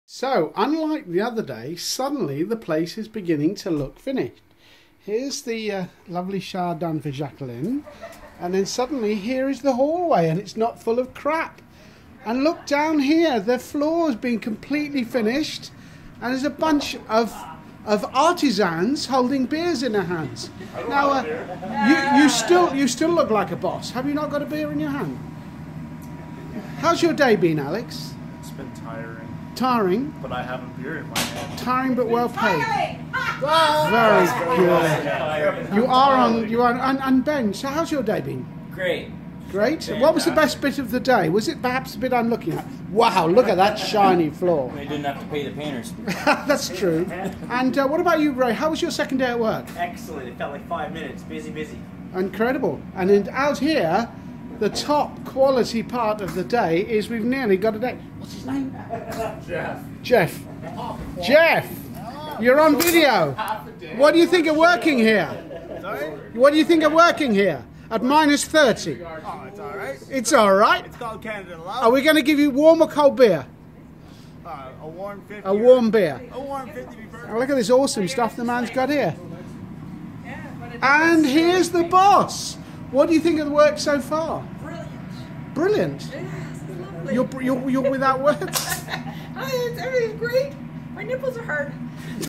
Skilbilt Day 52 - artisan & client interviews
Well earned beers after a long, hard, very cold week